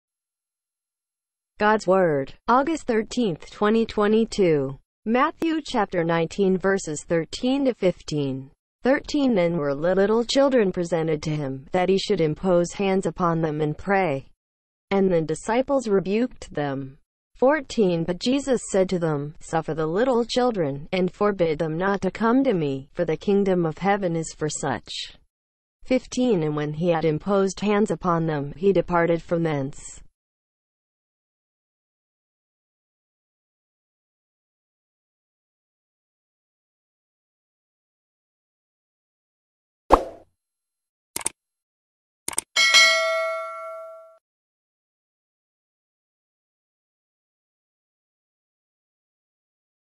Daily Bible Verse August 13, 2022 Matthew 19: 13-15 God's Word Bible Reading
13 Then were little children presented to him, that he should impose hands upon them and pray. And the disciples rebuked them.

14 But Jesus said to them: Suffer the little children, and forbid them not to come to me: for the kingdom of heaven is for such.

15 And when he had imposed hands upon them, he departed from thence.